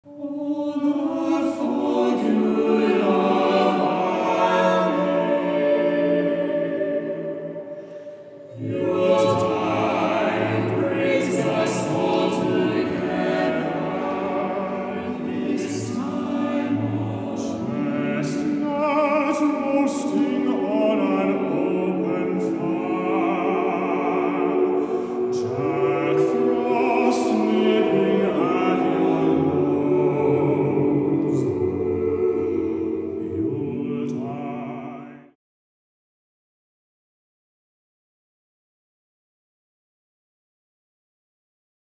Carols at the Museum 2020 trailer
A little preview of Carols at the Museum 2020, which will be broadcast online on 19th December at 7pm. Book your tickets at sestinamusic.com!